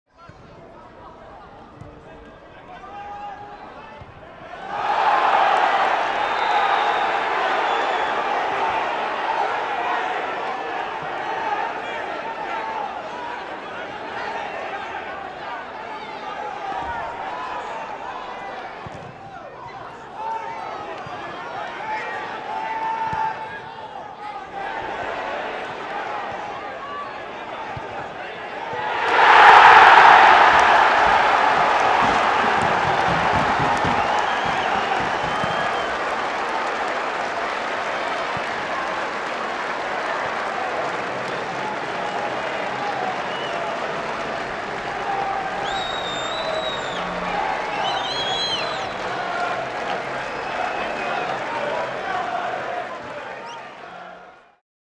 Unstoppable Andreu smashes in another goal
Hamilton goalscoring supremo Tony Andreu scores his tenth goal in 14 appearances this season as he hammers in an unstoppable volley to open the scoring during the 3-0 win over St Mirren at New Douglas Park in the SPFL Scottish Premiership.

This clip is from Hamilton 3-0 St Mirren, 22/11/2014